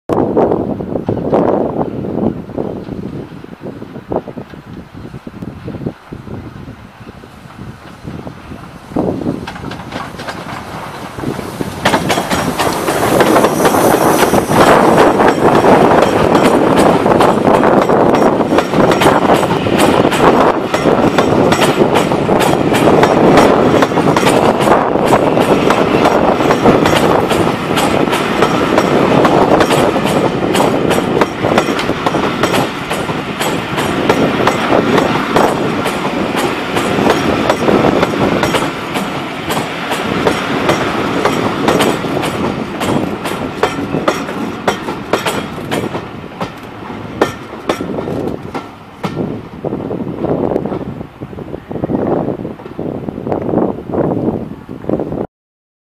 The No.35018 'British India Line' with The Winter Cumbrian Mountain Express was arrives at Carlisle.
The British Rail’s Ex-Southern Railway (Bulleid Pacific) Modified Merchant Navy Class 8P (Rebuilt) 4-6-2 No.35018 'British India Line' with The Winter Cumbrian Mountain Express from London Euston and he was now arriving at the Carlisle Citadel Station on Saturday 2nd March 2019